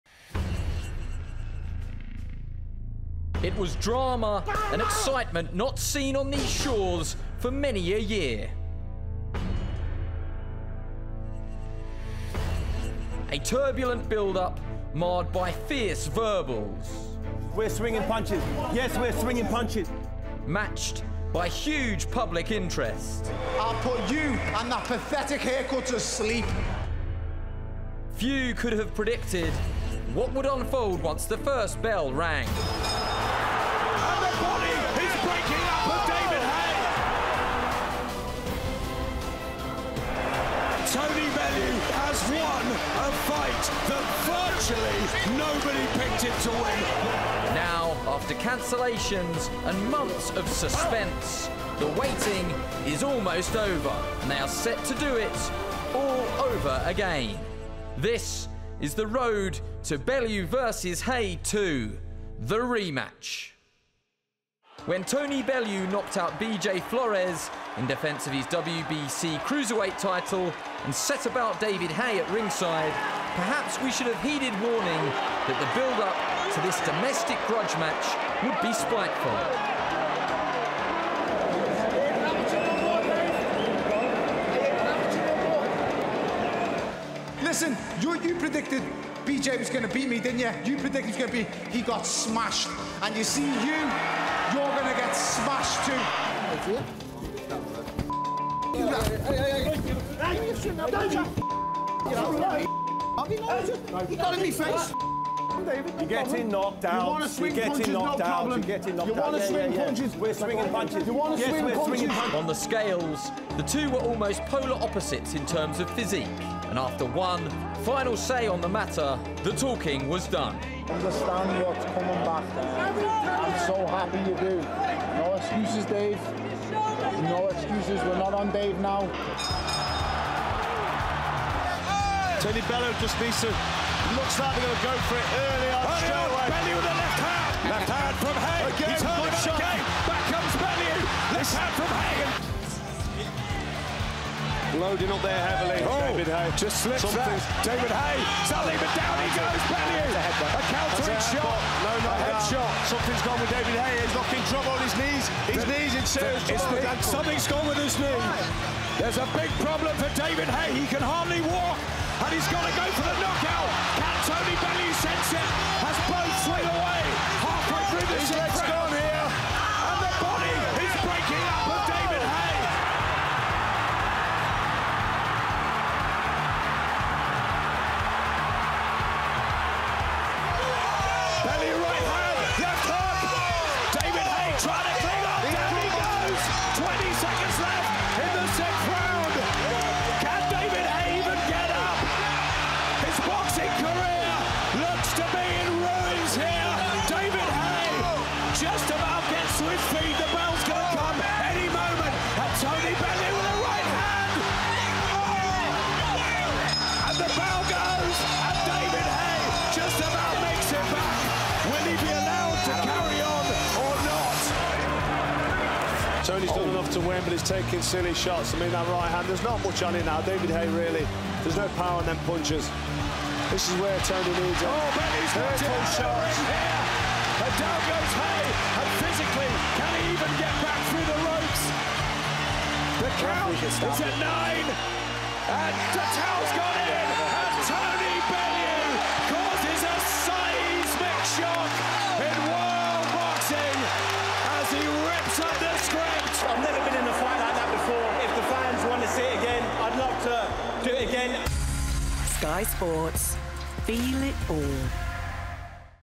The Road to Tony Bellew vs David Haye 2 | The Rematch | 5th May 2018
A look at the journey which has led to 'The Rematch' between Tony Bellew and David Haye on the 5th May 2018.